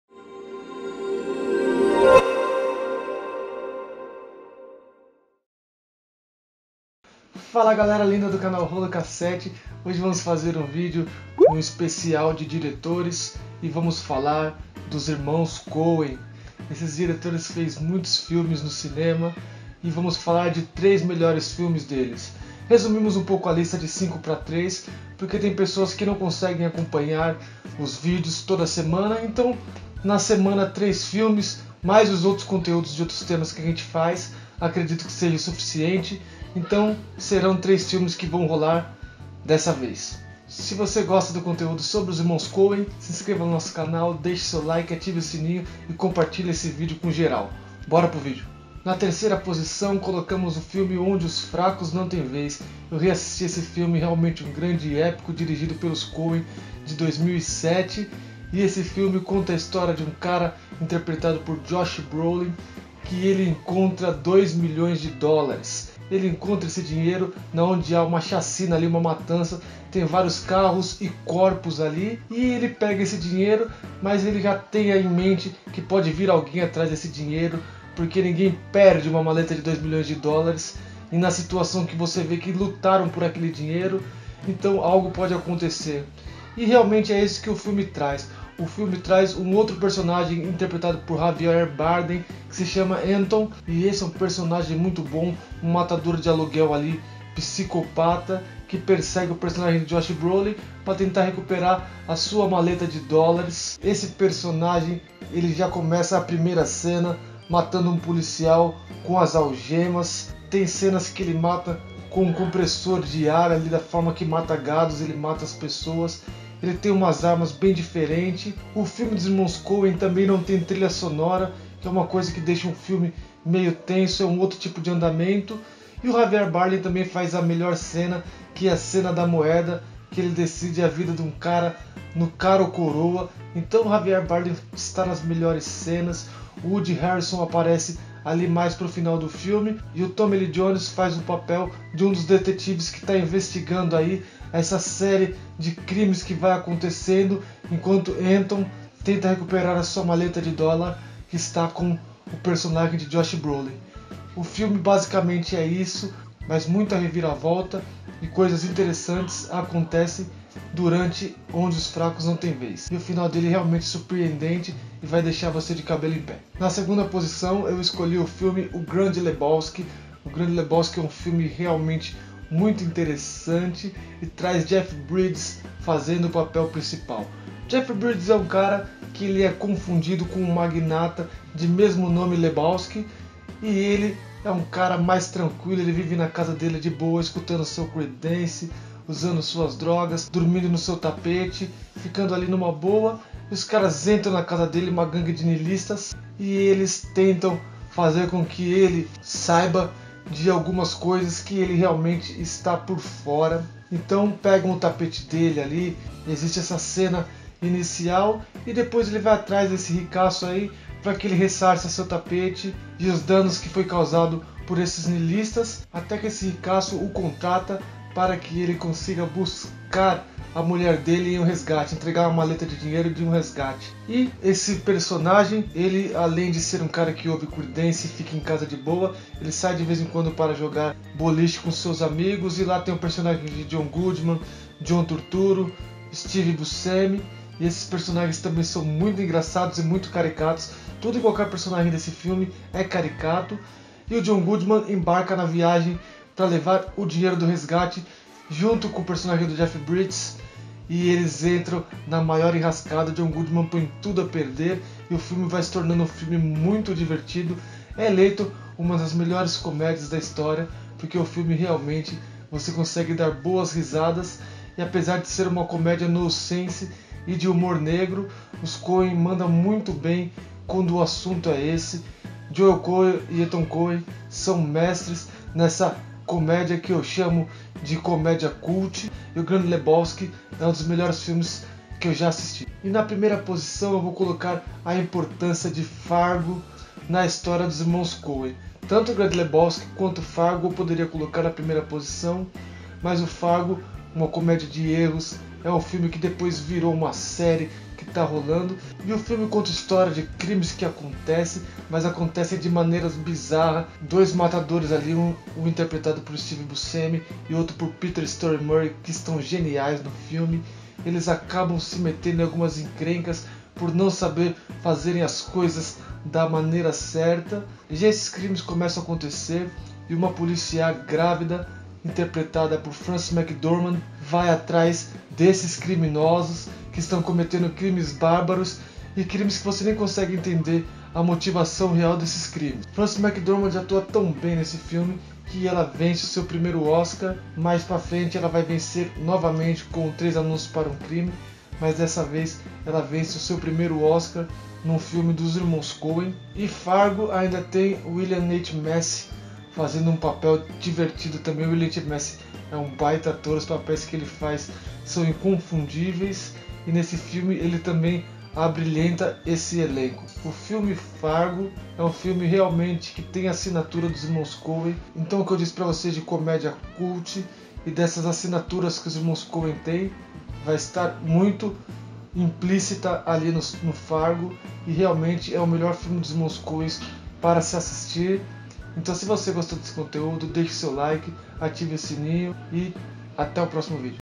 3 Melhores Filmes dos Irmãos Coen
3 Melhores Filmes dos Irmãos Coen
Os irmãos Coen já fizeram dezenas de filmes bons, Joel Coen e Ethan Coen trasz seus filmes com um humor peculiar e um drama interessante de se ver, neste vídeo destacamos 3 de seus melhores filmes Fargo, Onde os fracos não tem vez e O grande Lebowski.

TWEETER: @k7rolo